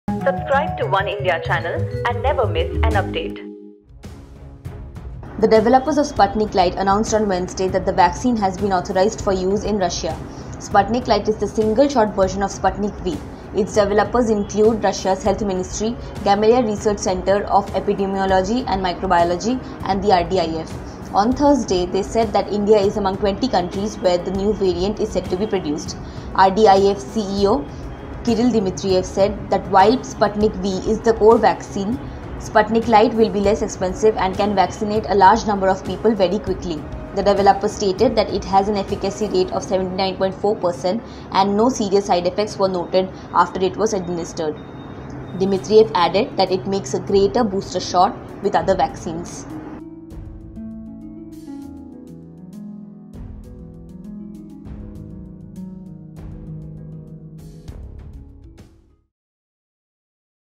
Russia's single-shot vaccine Sputnik Light gets the nod for use | Set to be produced in India
Sputnik Light is the single-shot version of Sputnik V. Its developers include Russia's health ministry, Gamaleya Research Center of Epidemiology and Microbiology,  and the RDIF. On Thursday, they said that India is among 20 countries where the new variant is set to be produced.